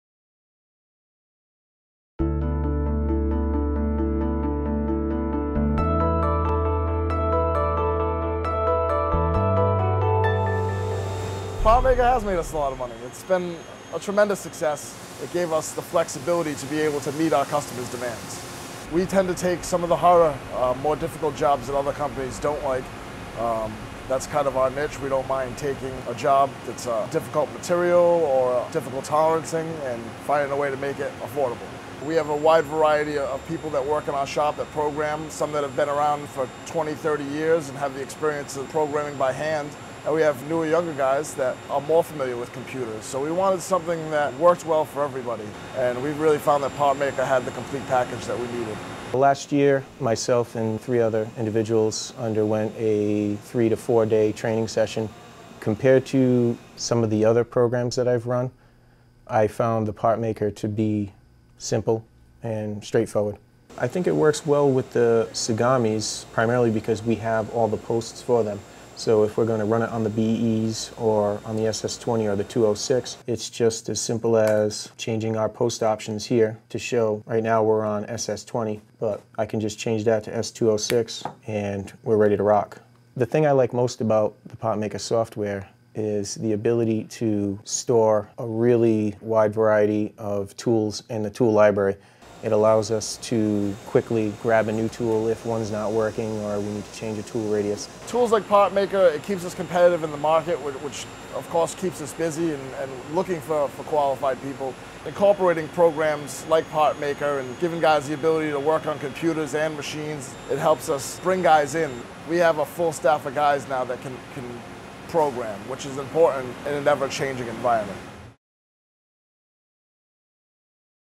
Differentiating with complexity with Autodesk PartMaker & Tsugami Machines at Marver Med
Programming Tsugami machines with Autodesk PartMaker enables Marver Med to take on more difficult jobs with challenging material and tolerance requirements.